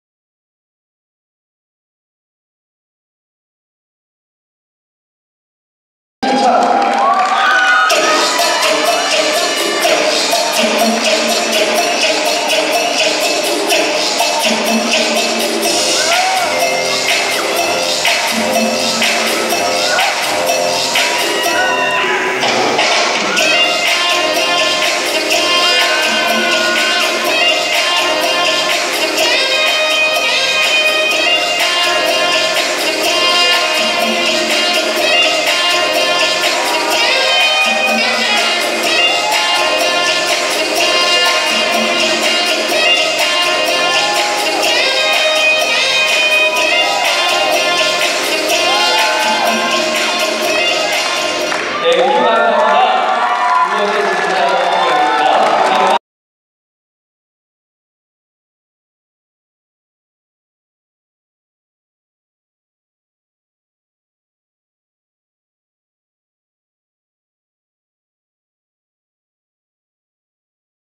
제1회 남양주시연합회장배 CEOCUP 전국 댄스스포츠 선수권대회 라틴프로 파이널 (차차차) 장세인이해인
제1회 남양주시연합회장배 CEOCUP 전국 댄스스포츠 선수권대회 라틴프로 파이널 (차차차) Latin professional Final (chachacha) 장세진 & 이해인 주최 : 남양주시체육회 주관 : 남양주시댄스스포츠연합회